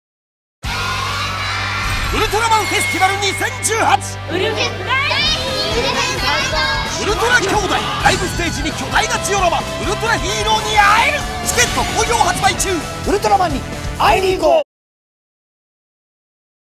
夏はウルフェス！「ウルトラマンフェスティバル2018」前売券発売中！
夏はウルフェス！!
今年のテーマは「ウルトラ兄弟の絆！」
ウルトラヒーローと一緒に、池袋・サンシャインシティで、怪獣・星人軍団 1,587体と戦おう！

ウルフェス初！ウルトラ兄弟VS怪獣・星人軍団 「大決戦！ 超巨大フォトスポット」登場！

ウルトラライブステージは、昨年大好評だった「ウルトラフラッシュ」を今年も全座席に設置！客席全体がヒーローと一緒に盛り上がる！
大人気のステージ前方事前予約席「ウルトラシート」は約220席（前年約120席）に拡大して販売！

大人気バトルジオラマもさらにパワーアップ。フォトスポットも盛りだくさん。
公式サポーターは爆笑問題さんに決定！

チケットご購入はこちら↓
セブンチケット